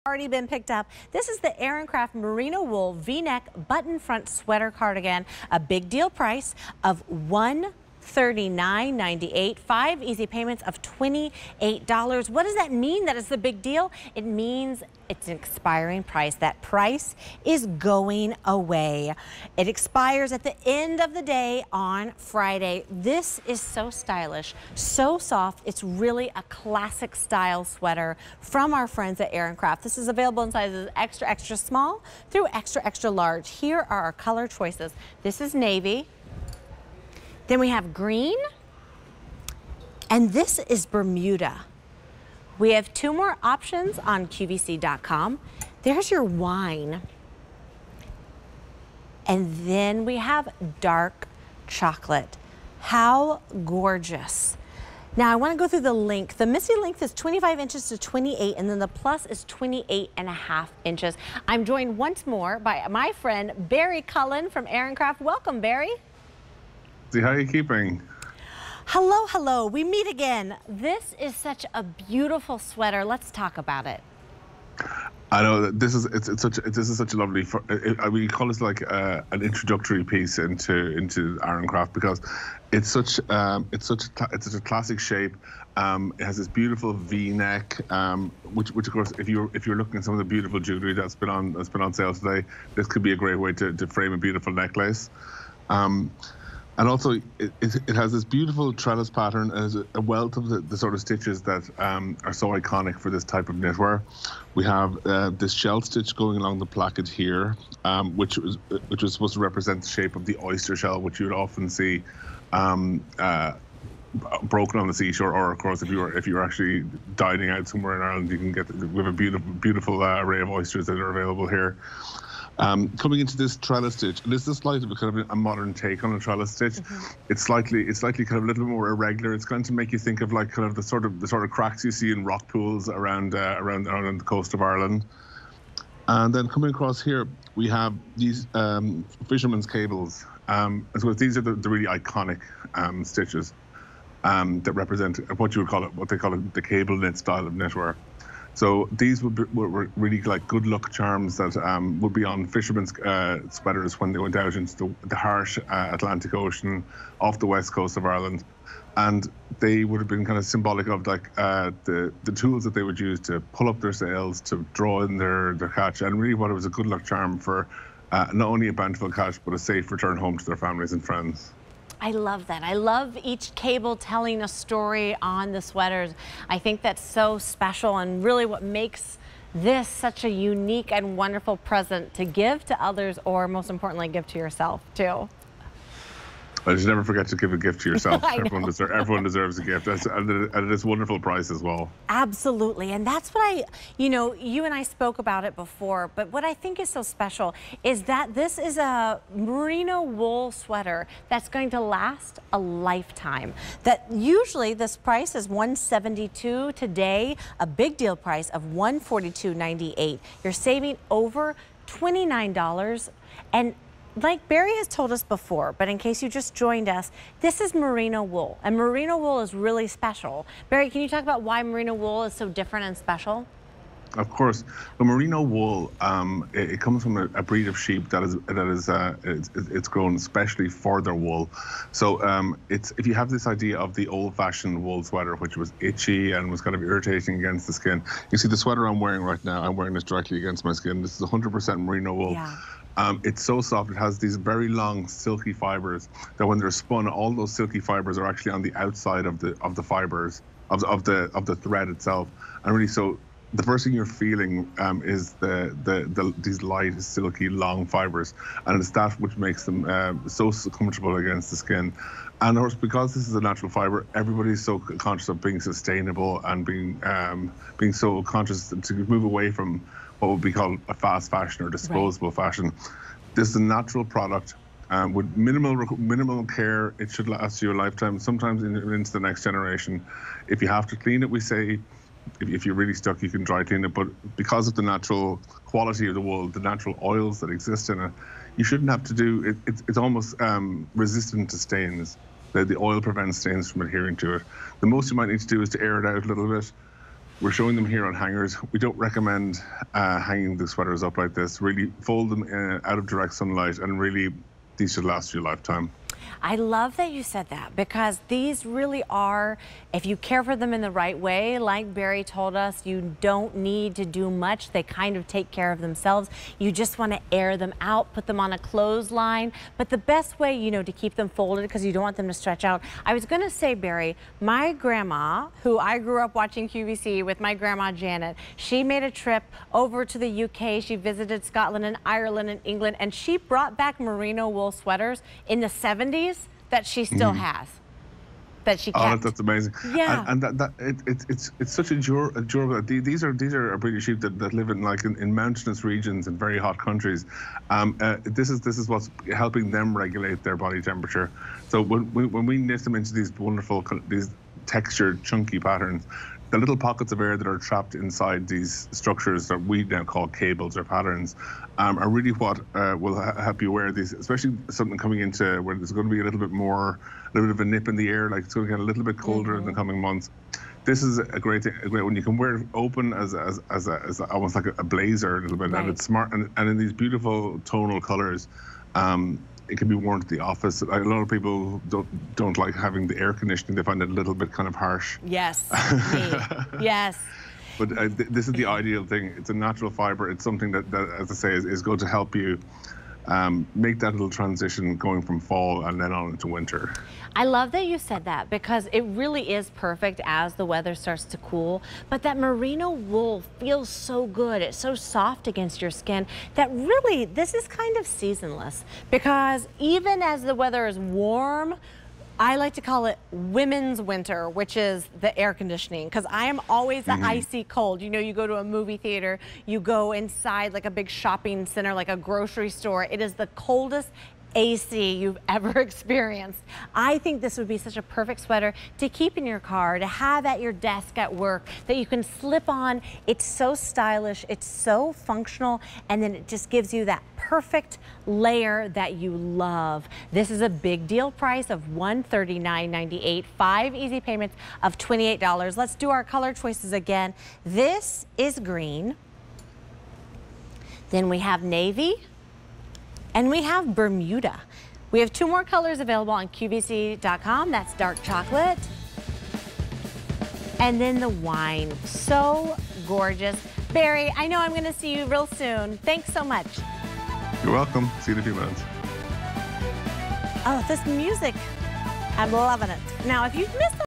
Aran Craft Merino Wool V-Neck Button Front Sweater Cardigan on QVC
Aran Craft Merino Wool V-Neck Button Front Sweater Cardigan

Crafted in Ireland from soft merino wool, this button-up cardigan combines classic cable knit and ribbed details with thoughtful design touches (love the dyed-to-match buttons!) to give you a wonderfully warm layering option that you'll reach for again and again. From Aran Craft.